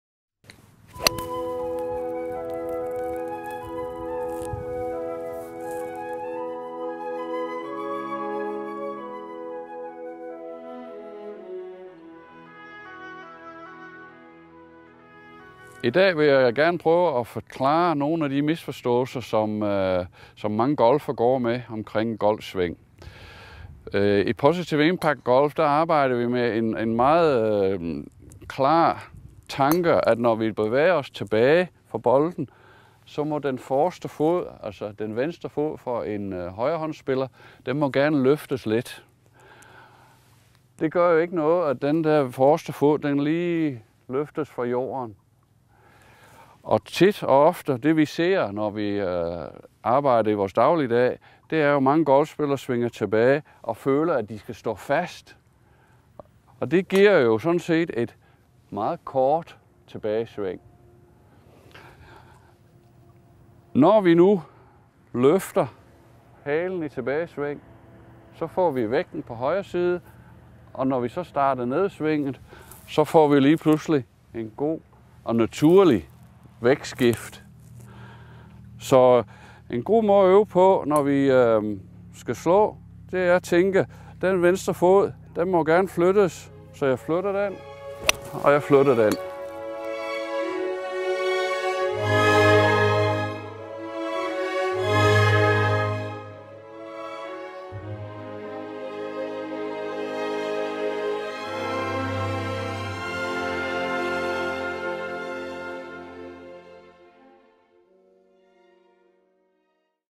Positive impact golf: løft føddderne
Ian viser hermed et tip til en bedre balance